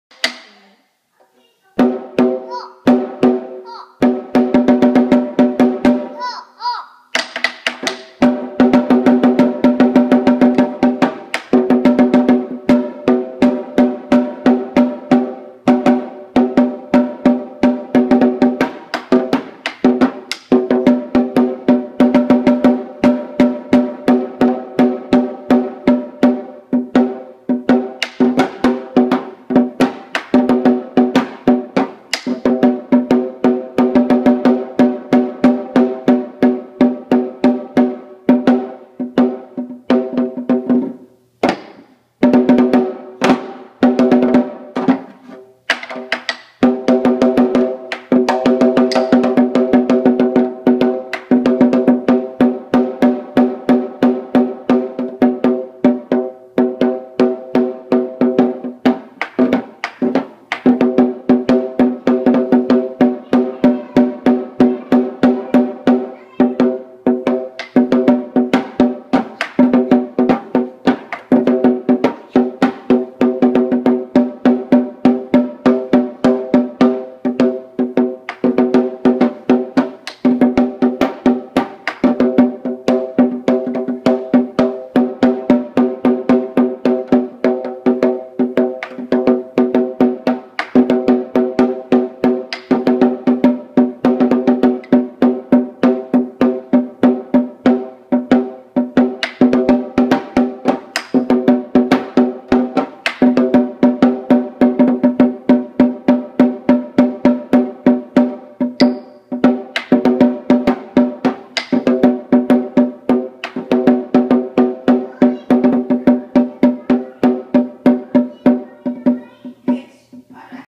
🥁 🦁 3yr old Tyler testing out his new lion dance drum
Tyler got his lion dance drum.